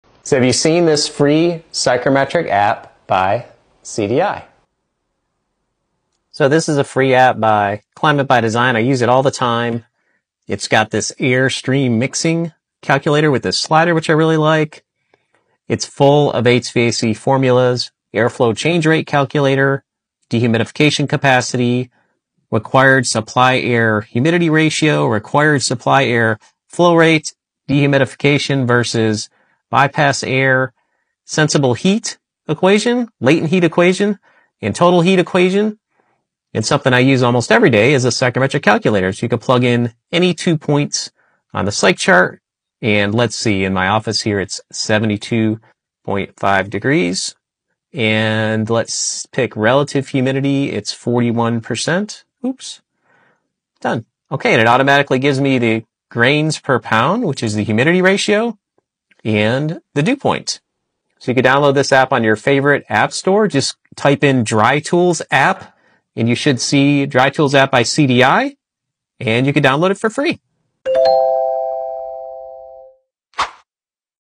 Free HVAC app called Dry Tools
Free HVAC app called Dry Tools. Mixed air calculator, properties of air and dozens of useful hvac formulas. Download in your app store. hvac hvactechnician hvaclife @ClimatebyDesignInternational